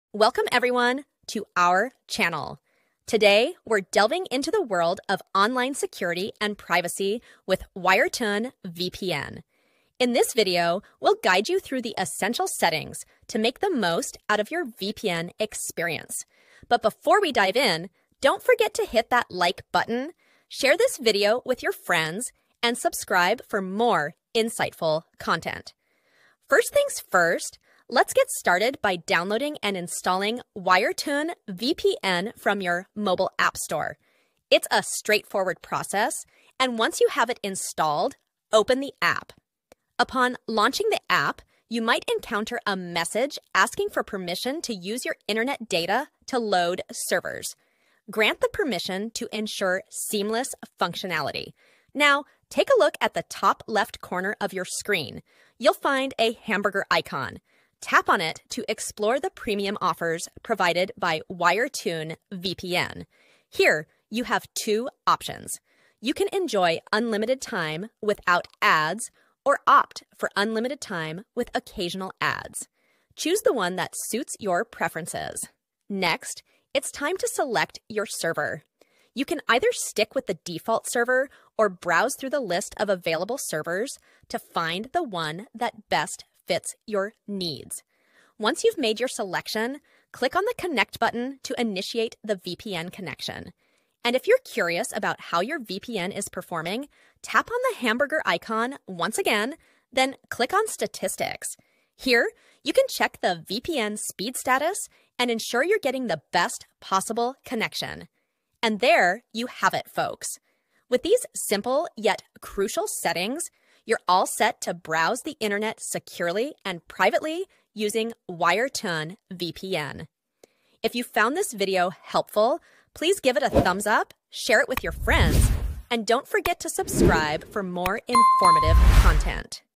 Wire TUN VPN All Settings
In this video, we'll walk you through the essential settings of Wire TUN VPN, ensuring you have a secure and private internet browsing experience. Learn how to download, install, and configure the app for optimal performance.